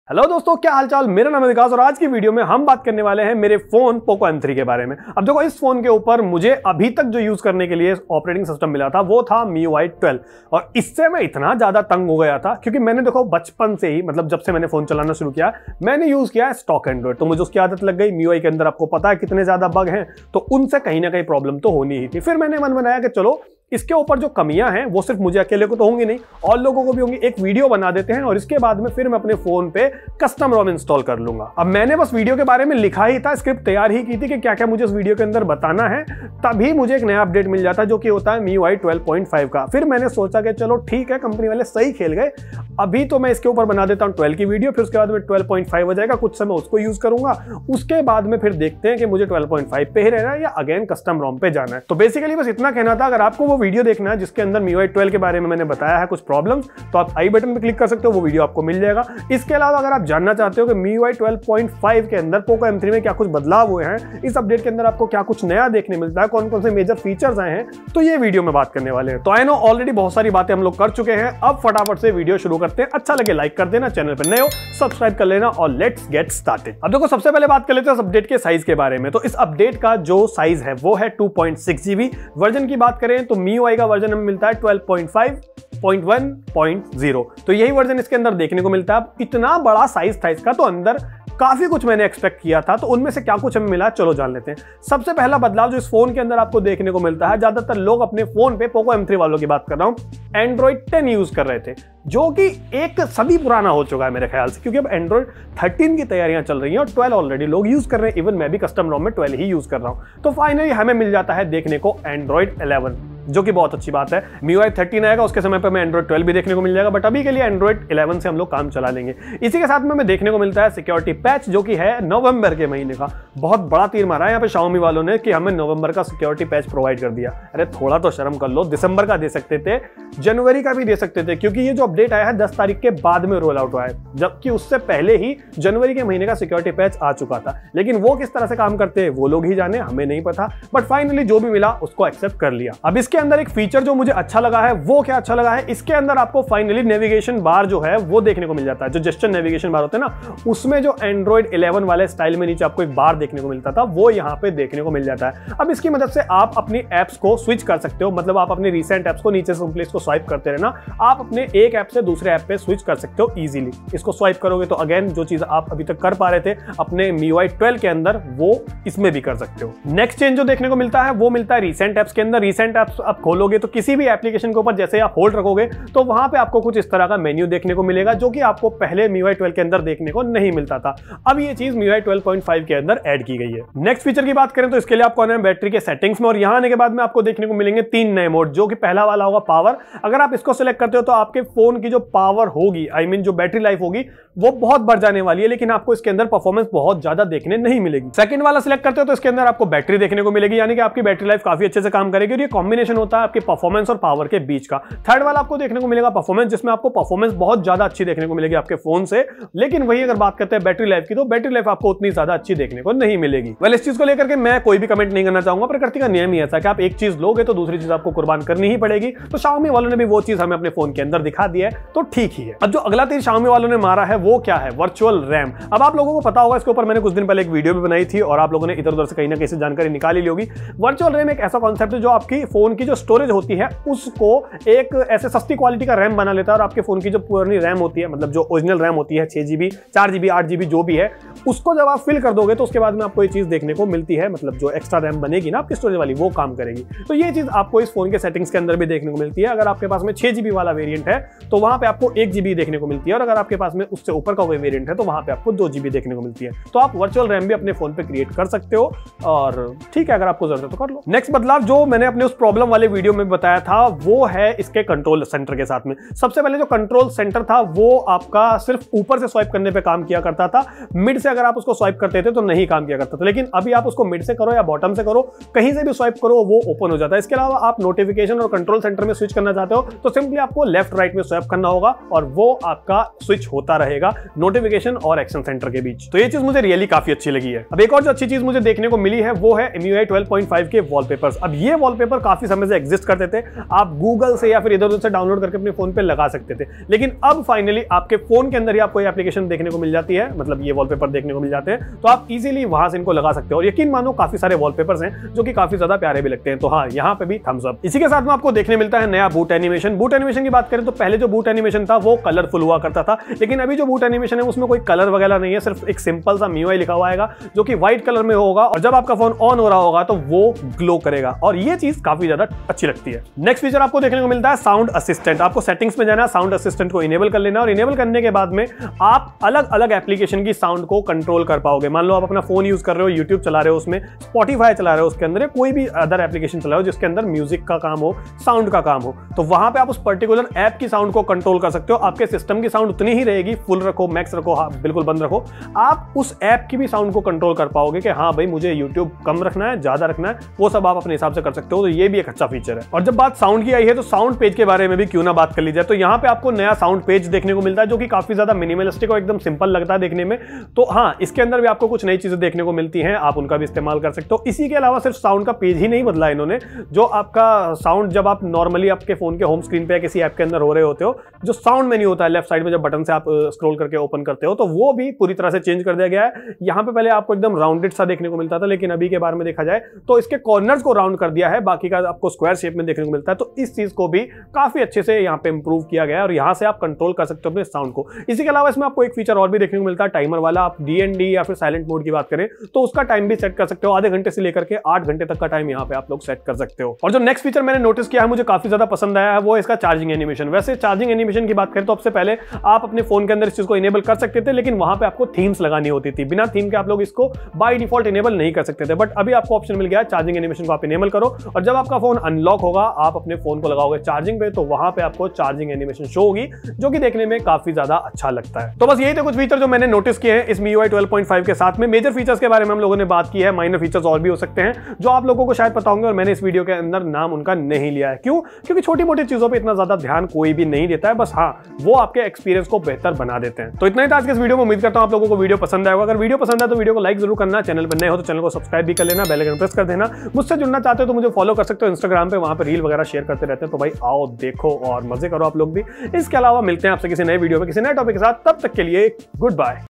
POCO M3 MIUI 12.5 [Android 11] Update is Here With Lots of New Features🔥🔥
POCO M3 MIUI 12.5 Android 11 Update Review | New Features🔥🔥: Aaj ki video mein MIUI 12.5.1.0.RJFINXM Stable build ka review kiya hai. Jiske aapko Android 11 aur sath me kuchh new features bhi milte hai. Inhi features ke baare mein is video mein baat ki hai. Mujhe ummid hai aapko ye video pasand aayi hogi..

About: I'm Vikas Singh (Geeky Vikas), and here I will provide you quality videos on Android customization.